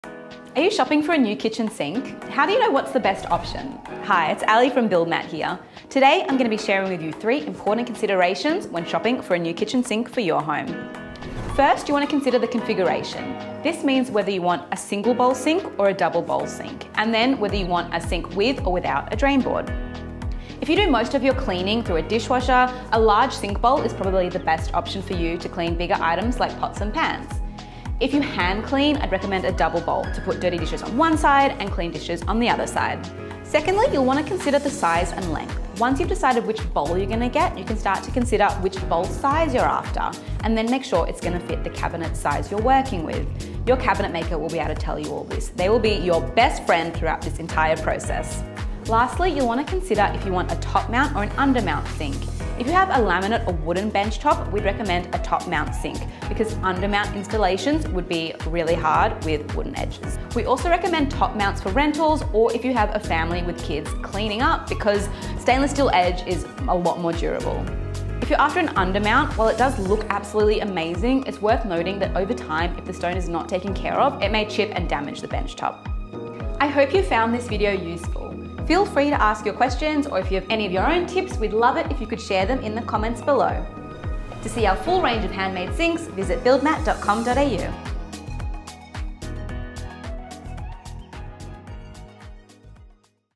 How to choose the best kitchen sink for your home?
Shopping for a new kitchen sink? Here are the top considerations:

1. Single Bowl or Double Bowl?
Single bowls are great if you clean most items in a dishwasher so you can wash larger pots and pans in the sink.
Double bowls are great if you hand clean your dishes, so you can soak the dishes on one side, and rinse on the other.

2. Kitchen Sink Size
This depends on the space you have to work with. Check with your cabinet maker!

3. Installation and Mounting
Wooden or laminate benchtop? Go with a top mount because undermount is usually quite difficult.
Stone benchtop? Undermount will look great, but we recommend top mount because it'll protect the benchtop from chipping as the sink will absorb the small dings through daily use.